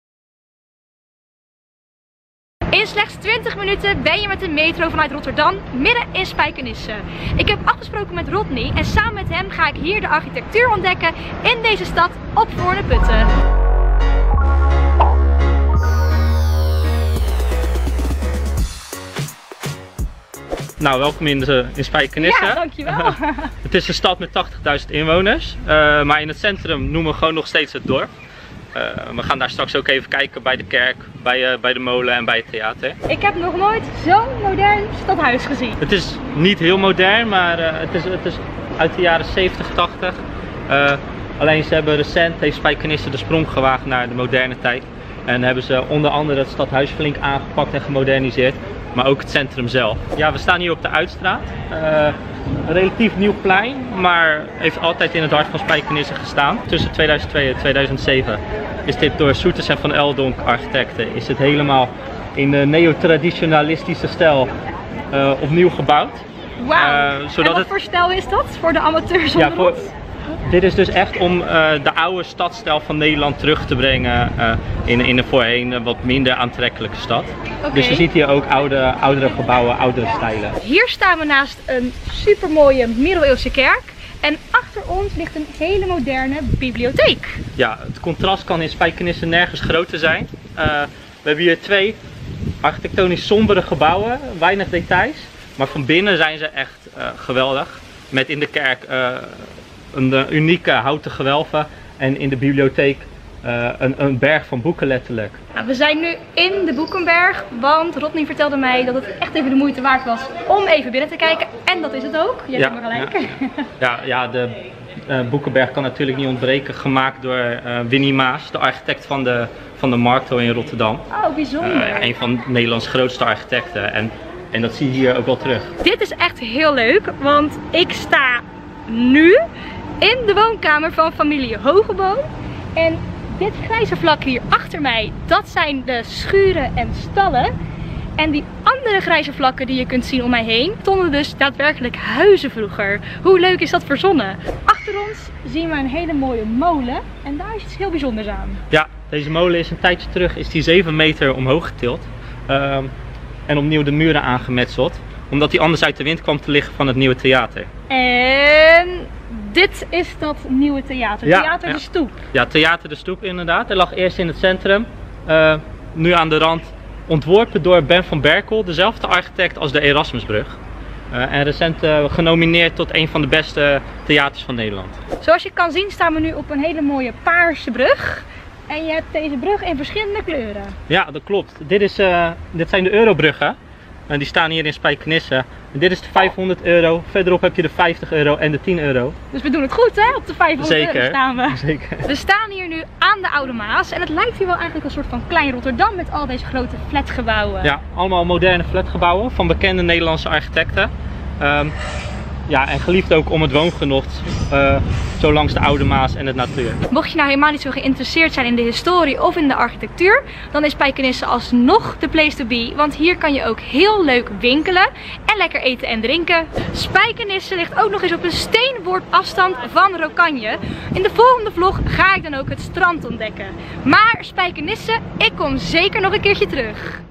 Vlog: Spijkenisse is een geheim paradijs van architectuur
Spijkenisse is een stad die groot is geworden door klein te blijven. Ondanks de 80.000 inwoners noemen inwoners hun binnenstad nog steeds gewoon het dorp. Toch is er een architectuur in Spijkenisse waar menig grotere stad jaloers op zal zijn.Vlogster Lieke gaat langs bij gebouwen als het stadhuis, de Middeleeuwse dorpskerk en de hypermoderne bibliotheek: de Boekenberg.